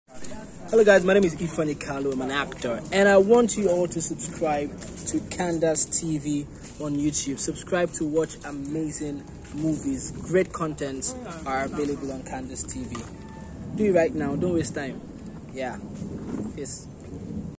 Actor Ifeanyi Kalu explaining his good experience with Kamdaz TV
you don't know what we are bringing to the table soon